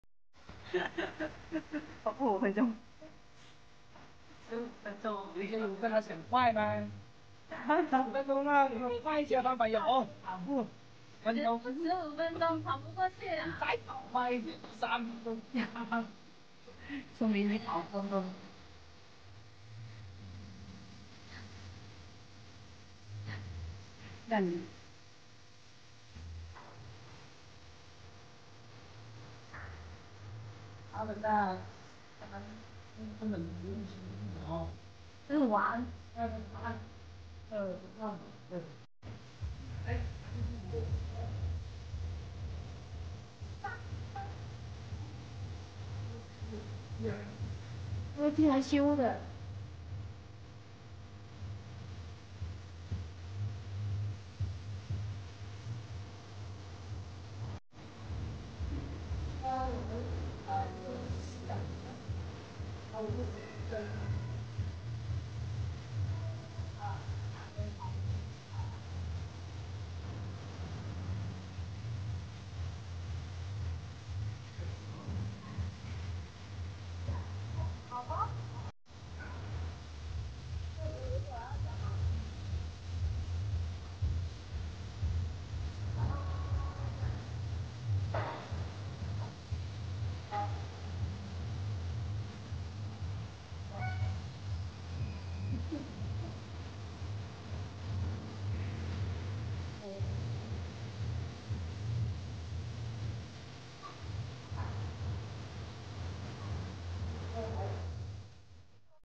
注意看这是一只快乐的溜达风，还有抢戏的卷子威武，淡定干饭的叽咕
大象,小象,亚洲象